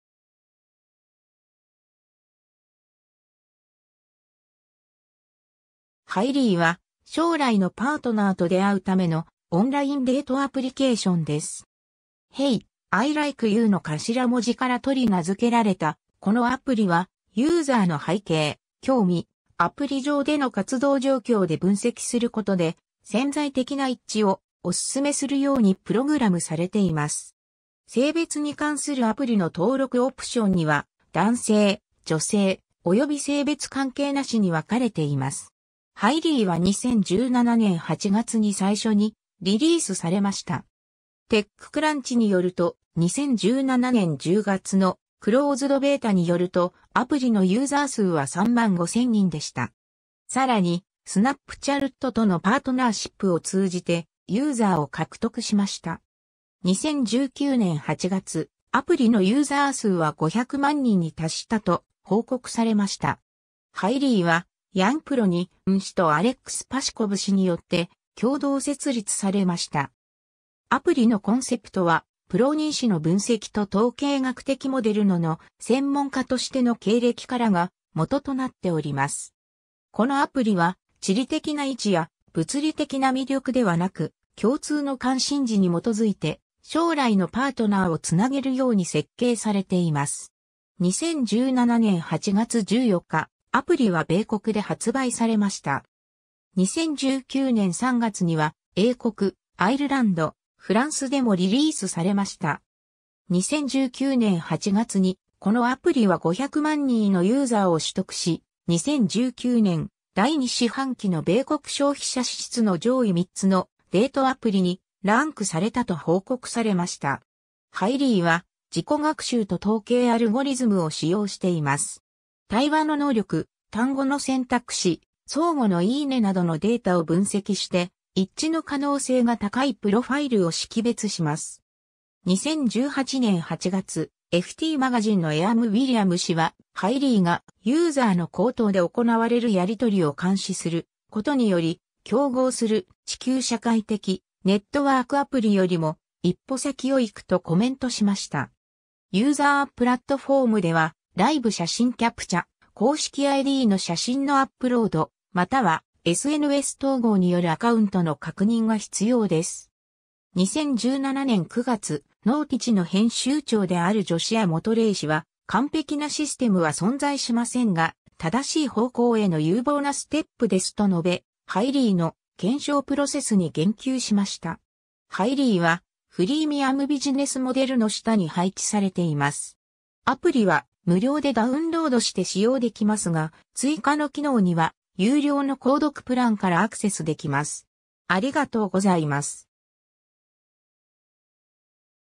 Hily
Hilyは、将来のパートナーと出会う ためのオンラインデートアプリケーションです。
 「Hey、I Like You」の頭文字から取り名付けられたこのアプリは、ユーザーの背景、興味、アプリ上での活動状況で分析することで、潜在的な一致をお勧めするようにプログラムされています。
 性別に関するアプリの登録オプションには、男性、女性、および性別関係無しに分れています。
 Hilyは2017年8月に最初にリリースされました。
 TechCrunchによると、2017年10月のクローズドベータによると、アプリのユーザー数は35,000人でした。
 更にSnapchatとのパートナーシップを通じてユーザーを獲得しました。
 2019年8月、アプリのユーザー数は500万人に達したと報告されました。
 Hilyはヤン・プロにン氏 とアレックス・ パシコブ氏 によって共同設立されました。
アプリのコンセプトは、プロニン氏の分析と統計学的モデルのの専門家としての経歴からが元となっております。
 このアプリは、地理的な位置や物理的な魅力ではなく、共通の関心事に基づいて将来のパートナーを繋げるように設計されています。
 2017年8月14日、アプリは米国で発売されました。
 2019年3月には、英国、アイルランド、フランスでもリリースされました。
 2019年8月に、このアプリは500万人のユーザーを取得し、2019年第2四半期の米国消費者支出の上位3つのデートアプリにランクされたと報告されました。
 Hilyは自己学習と統計アルゴリズムを使用しています。
 対話の能力、単語の選択肢、相互のいいねなどのデータを分析して、一致の可能性が高いプロファイルを識別します。
 2018年8月、FT Magazineの エアム・ウィリアム氏は、Hilyがユーザーの口頭で行われるやり取りを監視することにより、競合する地球社会的ネットワークアプリよりも「一歩先を行く」とコメントしました。
 ユーザープラットフォームでは、ライブ写真キャプチャ、公式IDの写真のアップロード、またはSNS統合によるアカウントの確認が必要です。
 2017年9月、KnowTechieの編集長であるジョシア・モトレー氏は、「完璧なシステムは存在しませんが、正しい方向への有望なステップです」と述べ、Hilyの検証プロセスに言及しました。
 Hilyは、フリーミアムビジネスモデルの下に配置されています。
 アプリは無料でダウンロードして使用できますが、追加の機能には有料の購読プランからアクセスできます。